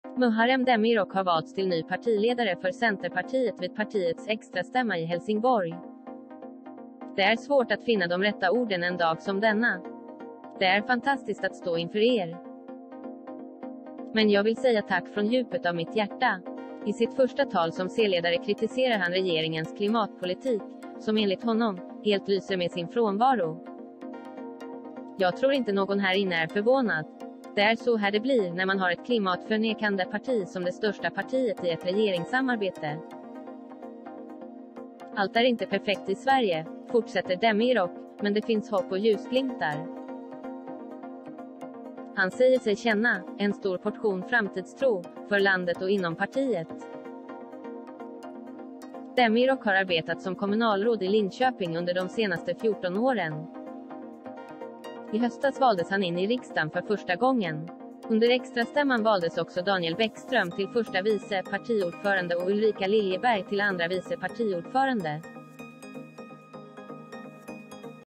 Demirok ny partiledare för Centern: ”Jag vill säga tack från djupet av mitt hjärta”
Muharrem Demirok har valts till ny partiledare för Centerpartiet vid partiets extrastämma i Helsingborg. – Det är svårt att finna de rätta orden en dag som denna. Det är fantastiskt att stå inför er. Men jag vill säga tack från djupet av mitt hjärta. I sitt första tal som C-ledare kritiserar han regeringens klimatpolitik, som enligt honom ”helt lyser med sin frånvaro”. – Jag tror inte någon här inne är förvånad. Det är så här det blir när man har ett klimatförnekande parti som det största parti